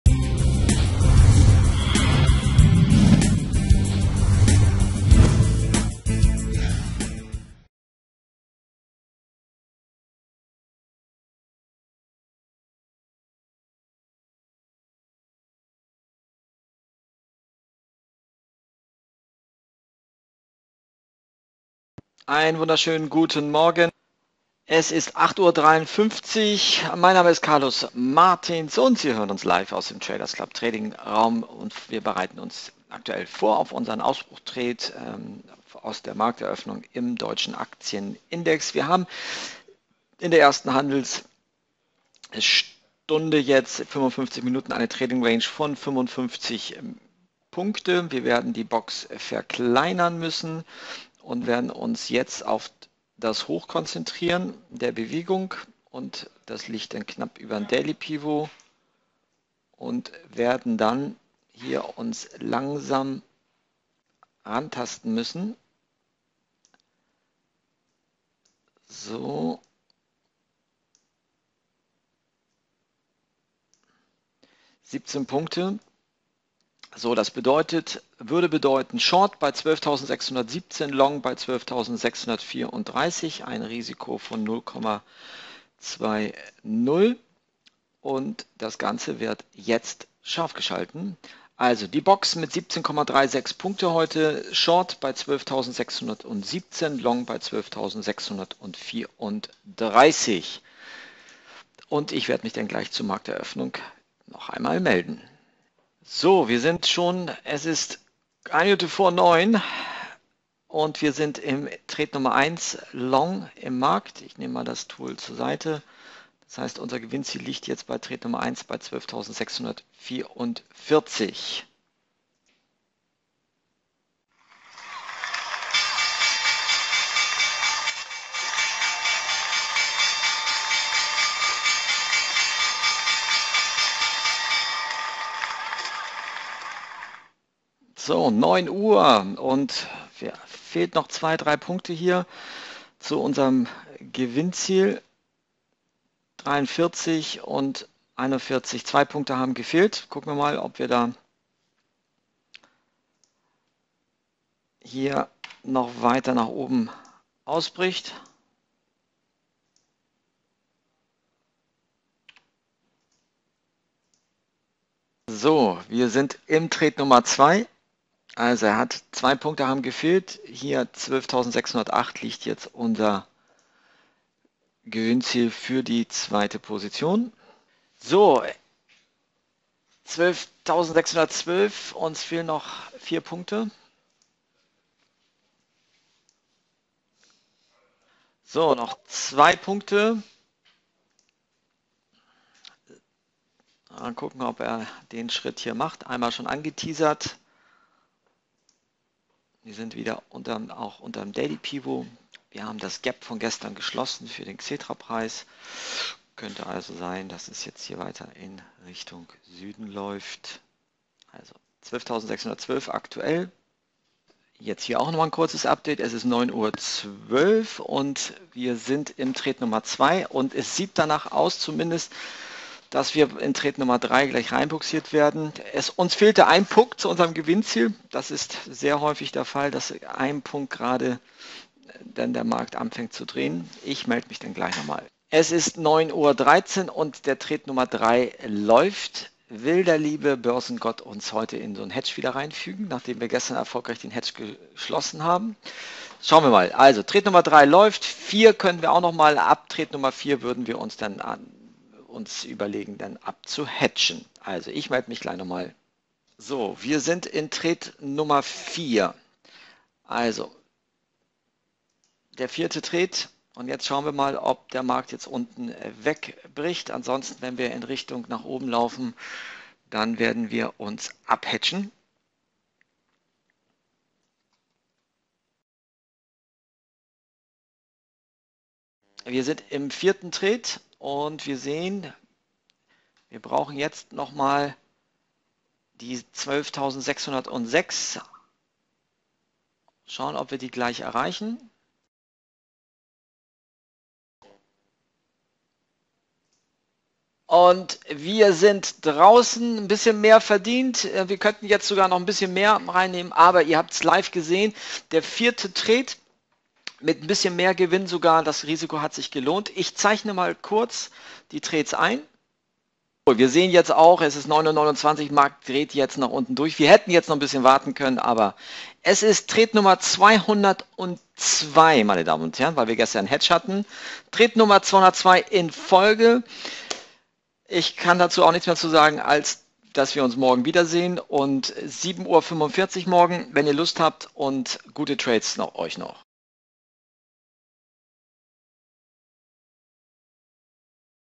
TC24 SwissBox OpenRange Breakout Live Trade Dax am 18.05.2017
Carlos Martins vom TradersClub24 zeigt die Anwendung des SwissBox-Trading mit einem OpenRange Breakout Live-Trade am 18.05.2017. Eine Demoversion können Sie herunterladen.